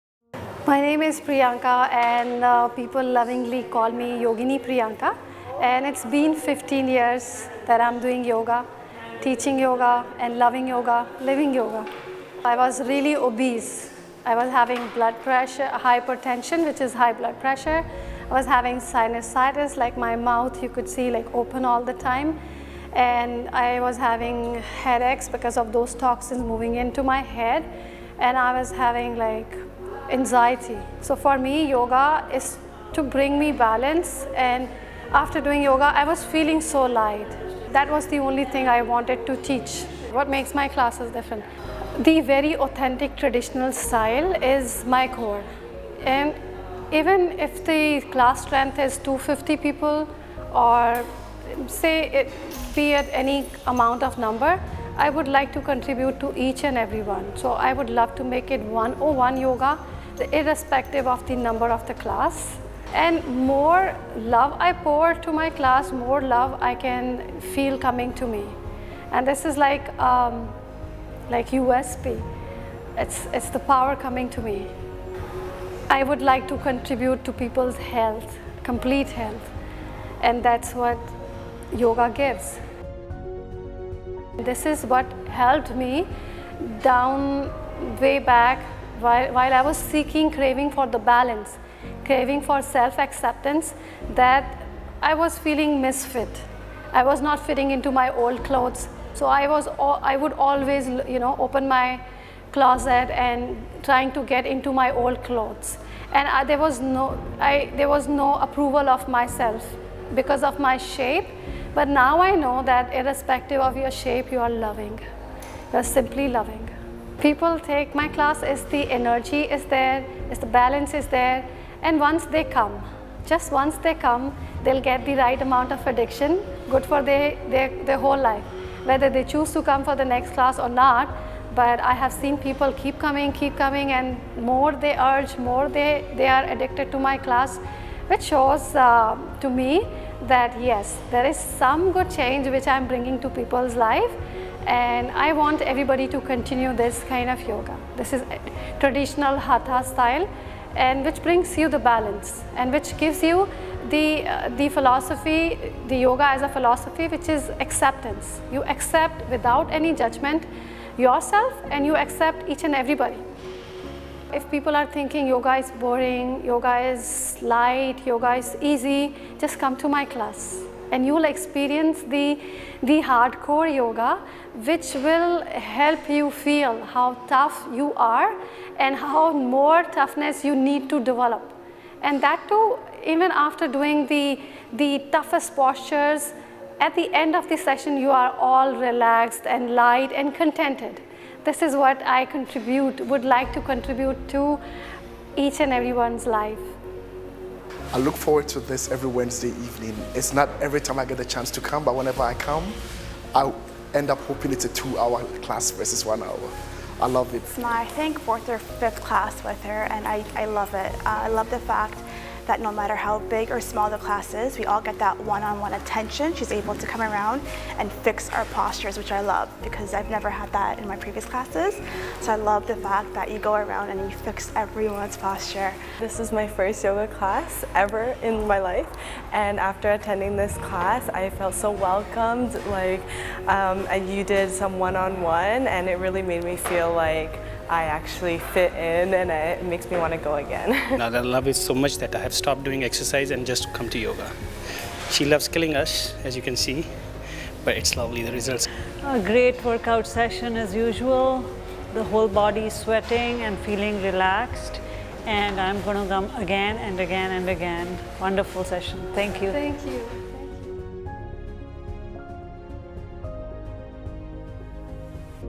My weight loss Journey - Transformation through yoga
I was 190 pounds before, not only obese but having high blood pressure, sinusitis and REAL low stamina. not only yoga helped me to shred the EXTRA pounds away but I had new HIGH levels of stamina. After doing yoga my life took a turn :) which was VERY pleasant. I urge you to try it once, through yoga you will come to know the beautiful you. Blessings and love !!!